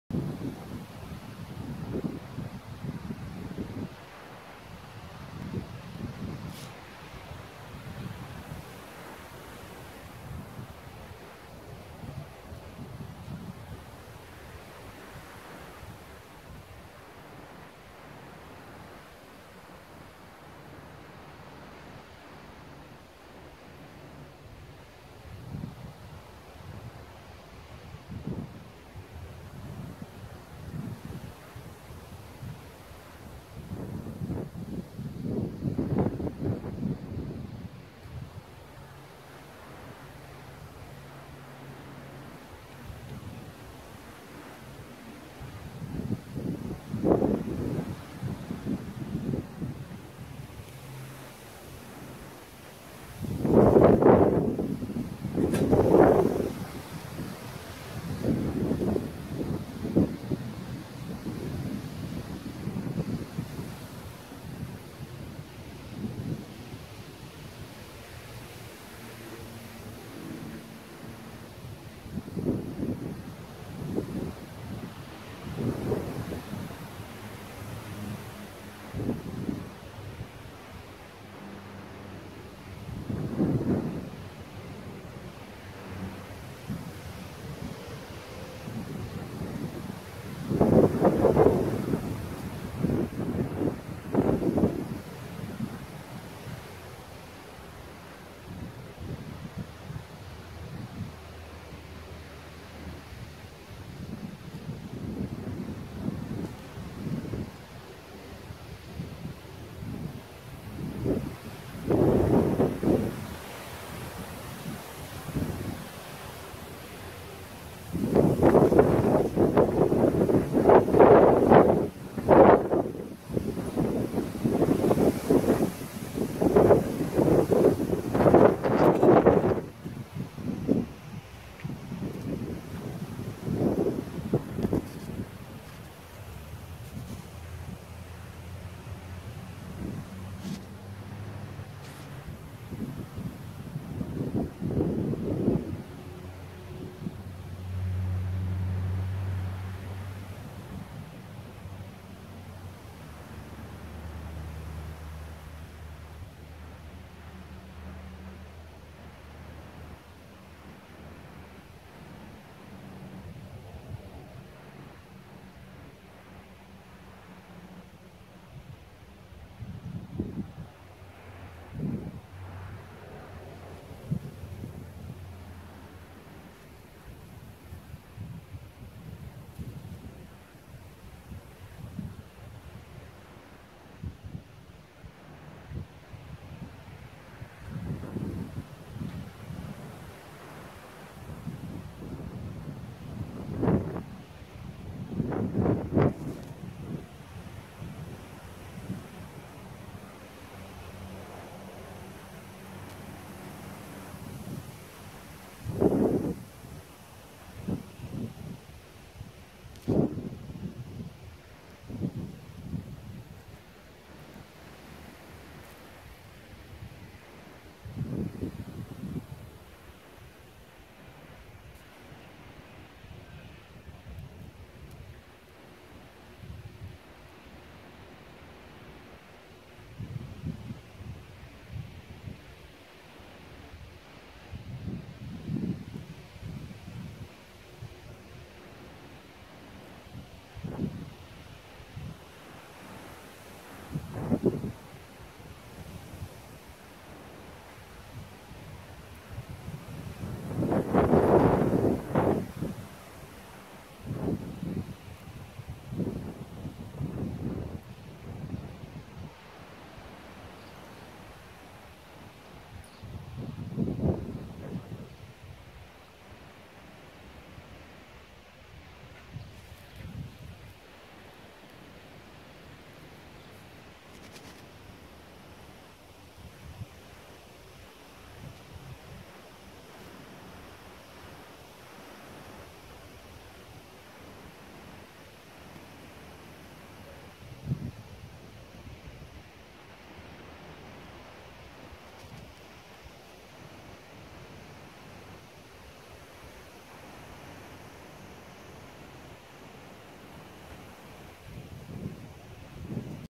First day of Summer 10.45am Blue Sun 😉
NE 🤔at 10.45🤔windy and storm rolling in fr. North 😉😎happy Summer 🤗